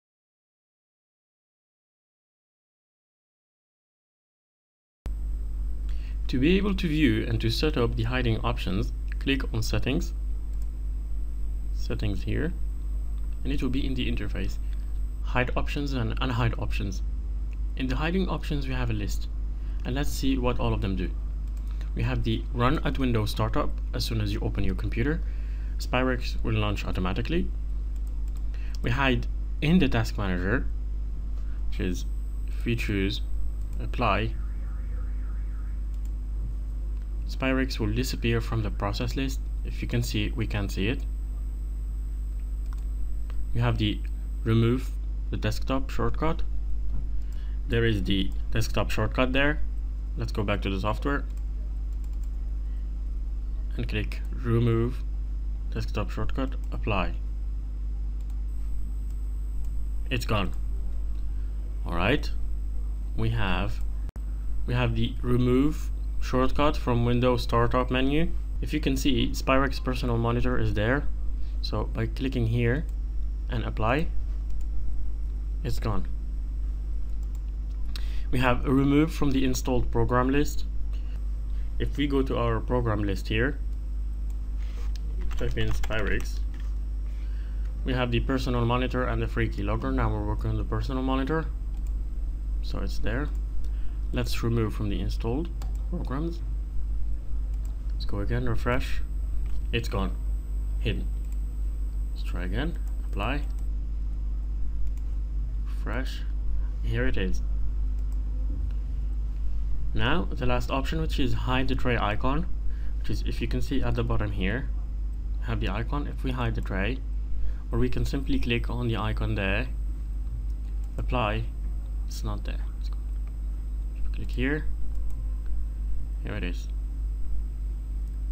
Spyrix Personal Monitor - hidden mode
Hide records of all activities on your PC: keystrokes (keylogger), passwords, screenshots, social networks monitoring including Facebook, MySpace, Google+